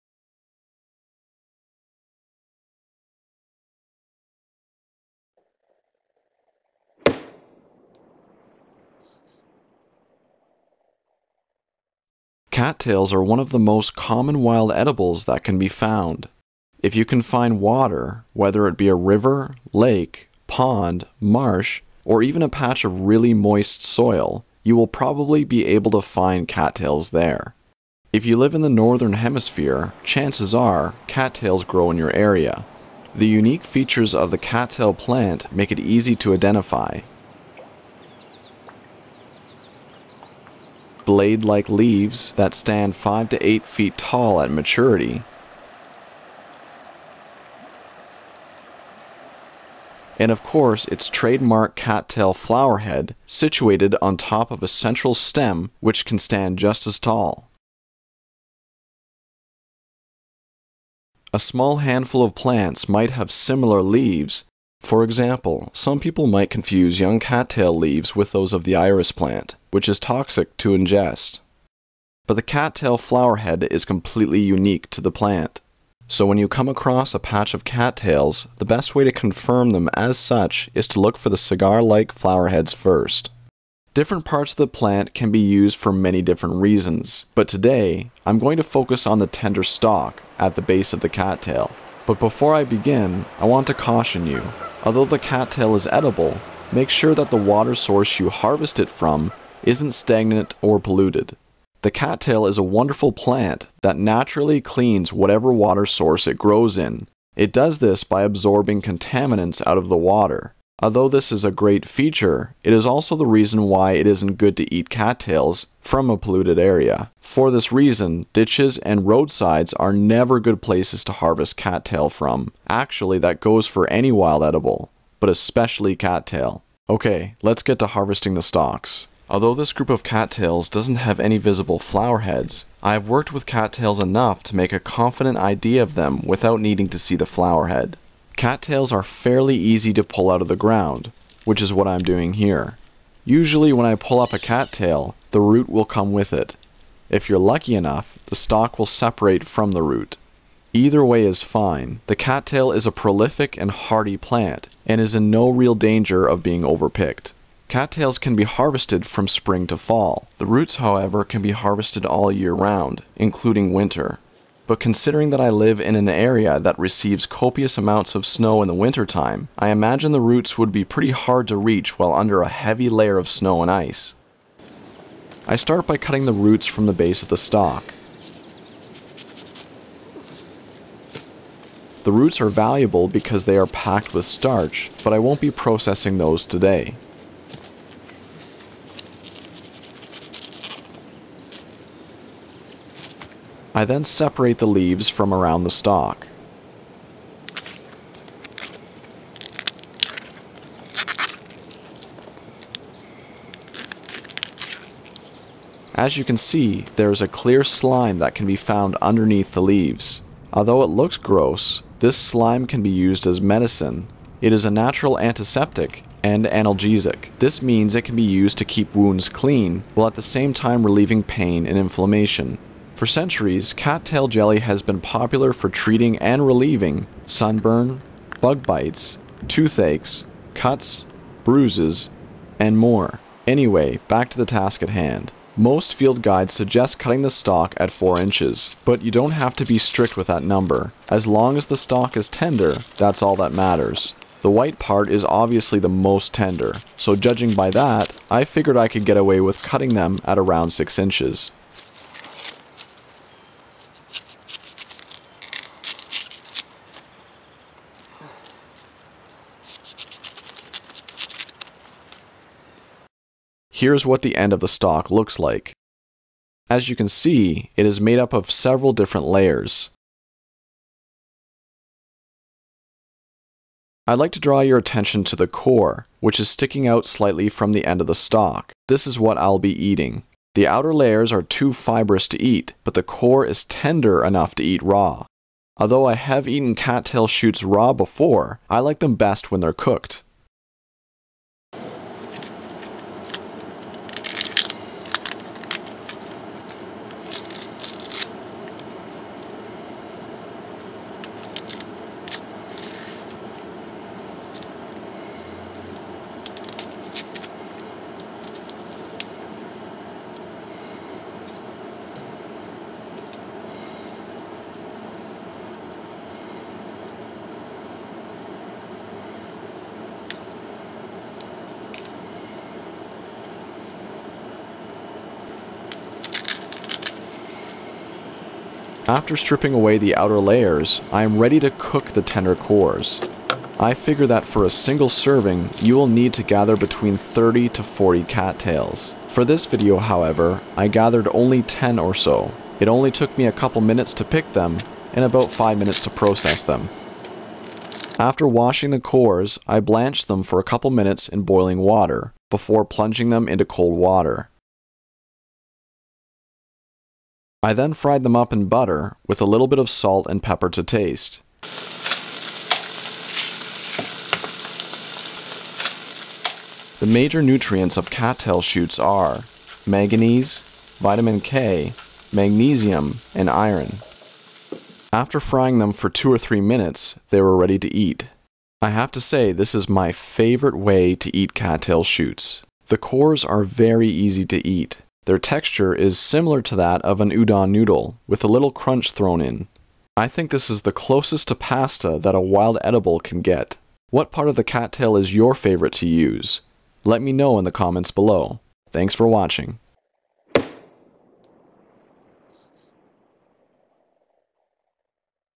Wild Food Foraging- Cattail- Veggie Pasta
In this episode, I identify the cattail plant, harvest the cattail shoots, and cook them. Their consistency is like that of an udon noodle. Could this be the next kind of vegetable pasta?

*Post-release update: In some states of the US, and in several other nations around the world, it is illegal to harvest cattail. Make sure to refer to your local laws first before picking cattails for yourself.